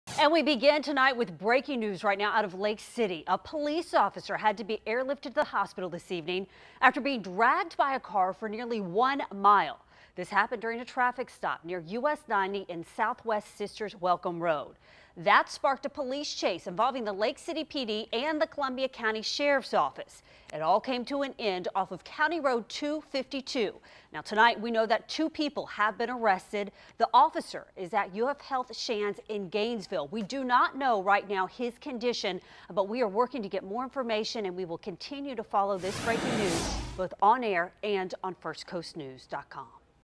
Lake City officer dragged by car following traffic stop
The officer has been airlifted to the hospital.